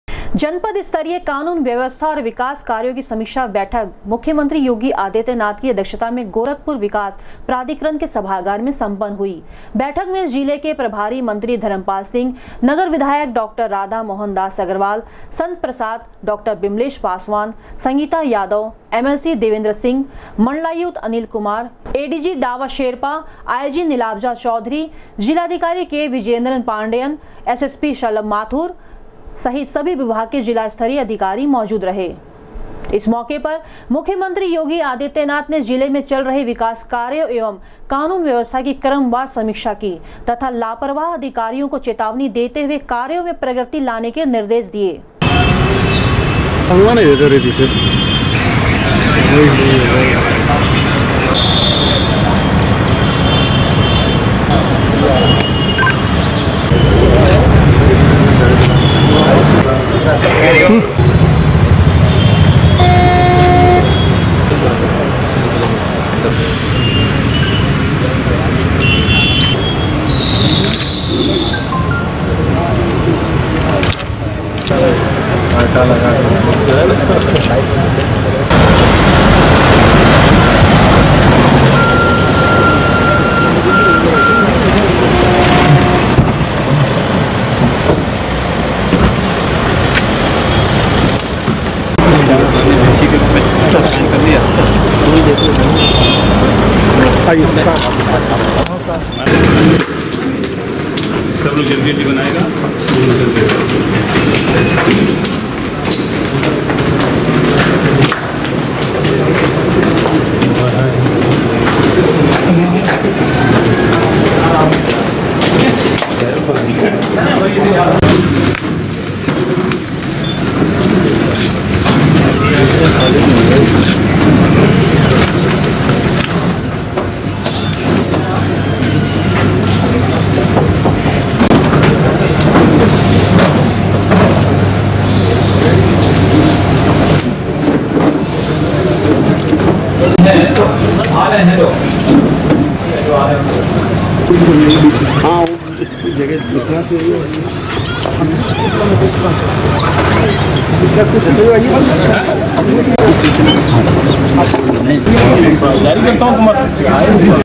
मुख्यमंत्री योगी आदित्यनाथ की अध्यक्षता में सम्पन्न हुई जनपद स्तरीय कानून व्यवस्था और विकास कार्यो
मुख्यमंत्री योगी आदित्यनाथ की अध्यक्षता में सम्पन्न हुई जनपद स्तरीय कानून व्यवस्था और विकास कार्यो की समीक्षा , लापरवाह अधिकारियो को चेतावनी देते हुए कार्यो में प्रगति लाने के दिए निर्देश